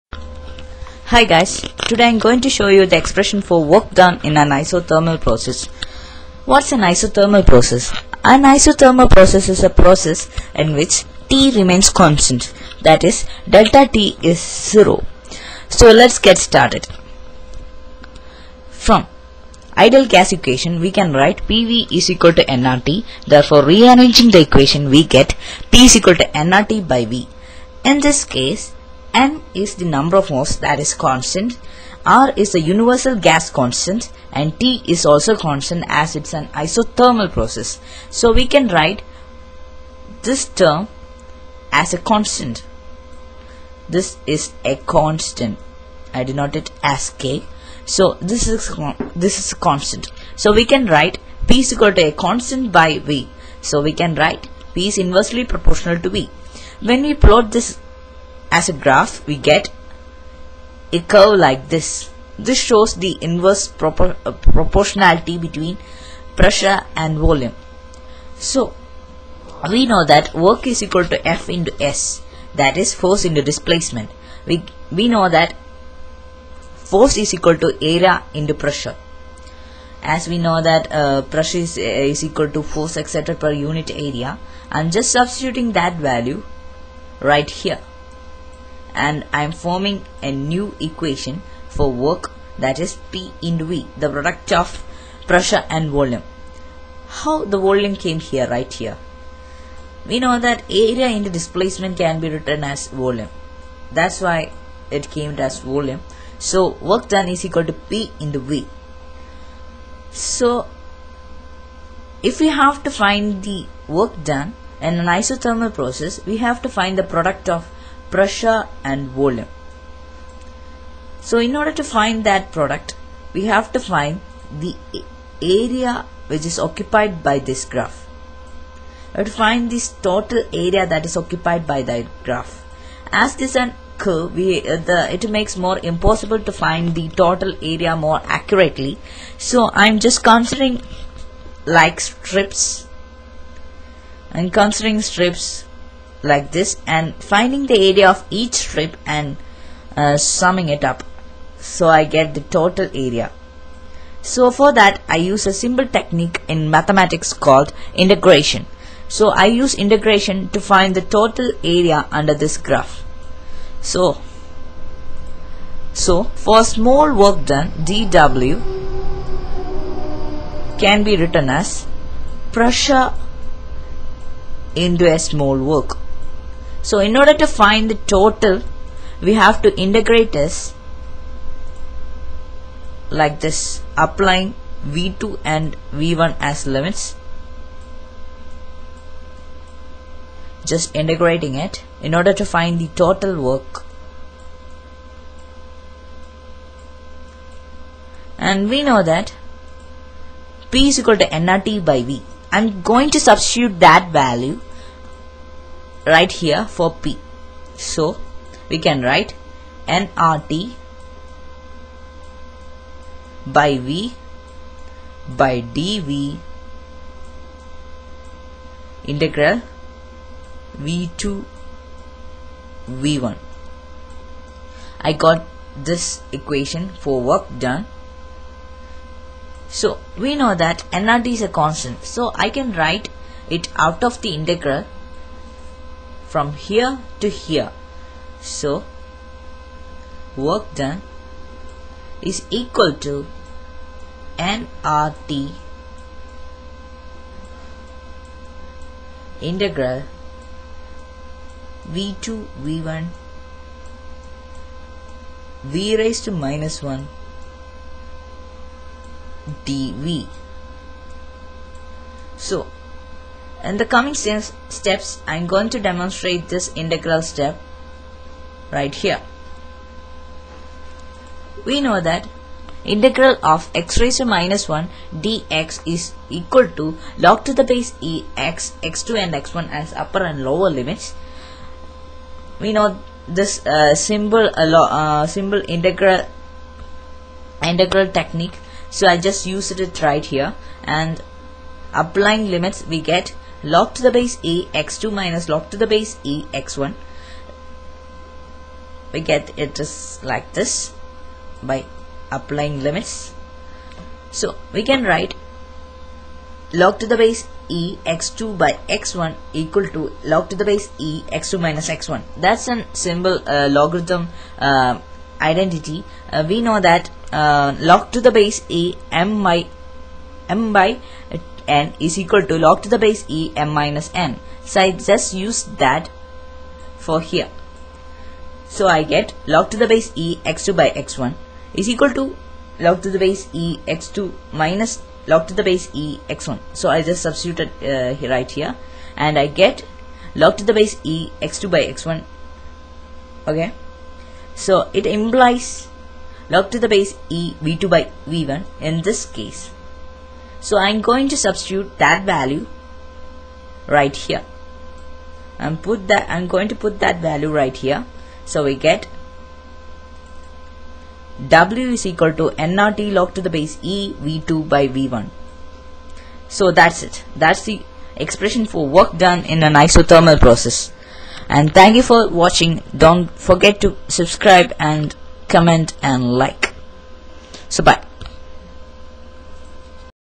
work done in an isothermal process
the derivation for the expression for the work done in an iso thermal process...